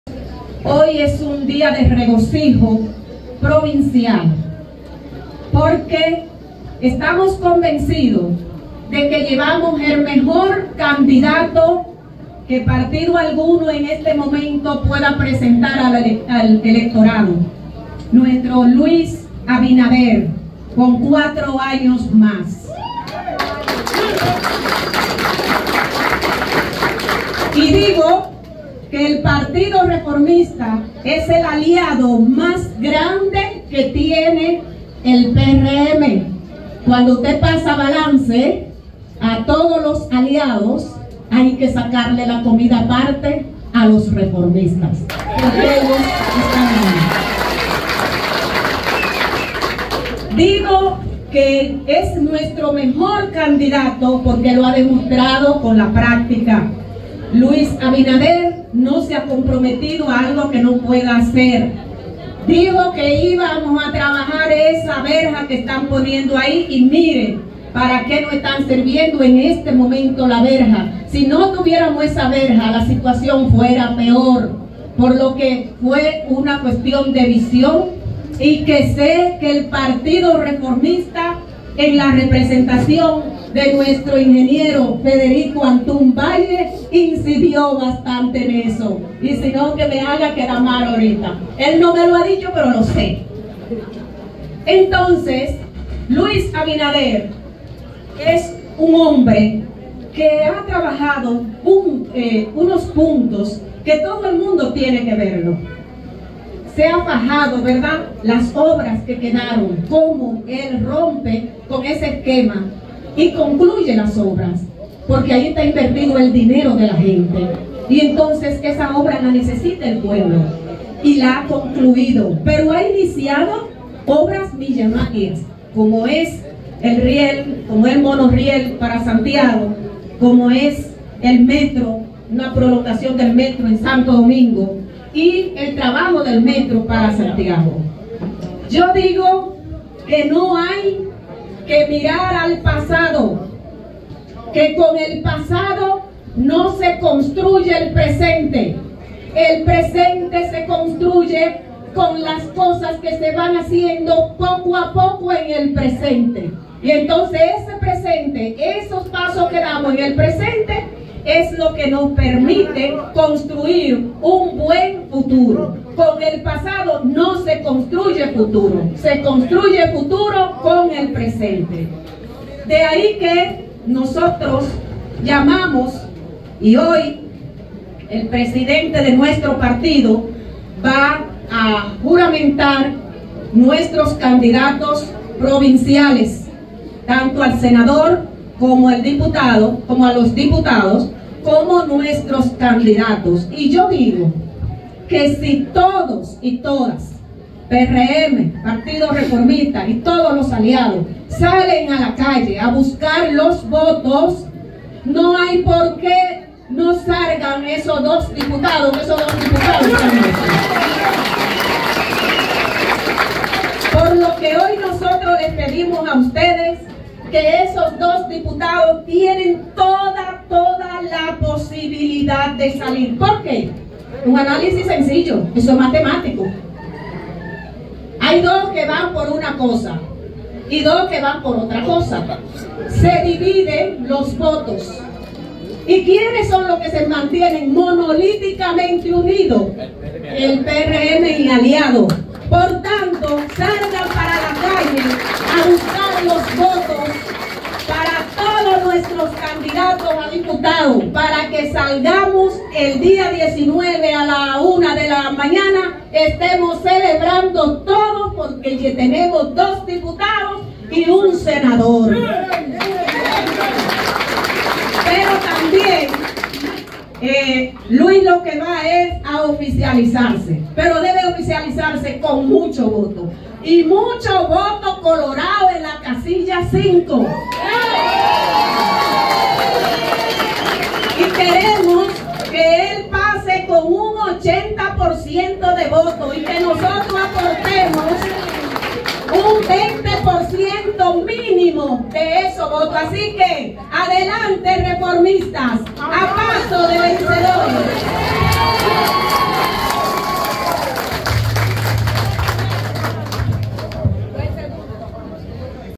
Primitiva Medina destaca la importancia del PRSC como aliado del PRM en las elecciones
La alta dirigente nacional del Partido Reformista Social Cristiano (PRSC), Primitiva Medina, reafirmó el papel crucial del PRSC como aliado del Partido Revolucionario Moderno (PRM), enfatizando la necesidad de reconocer su relevancia independiente en el panorama político.

Durante un acto de proclamación de candidatos congresuales en Elías Piña, Medina subrayó que el PRSC sigue siendo una fuerza significativa, destacando su compromiso continuo con la política nacional.

En sus declaraciones, Medina elogió al presidente Luis Abinader como la mejor opción para liderar el país en las próximas elecciones del 19 de mayo, instando a la población a votar por él en la casilla 5 para asegurar cuatro años más de su administración.

El evento, encabezado por el presidente del PRSC, Federico Quique Antún Batlle, contó con la presencia de candidatos congresuales del PRM, incluido Jhonoson Encarnación, candidato a Senador, y los candidatos a diputados Luis Castillo Ogando y Millys Martínez.

La intervención enérgica de Primitiva Medina subraya la importancia de la alianza entre el PRSC y el PRM, así como el respaldo hacia el presidente Abinader en las próximas elecciones.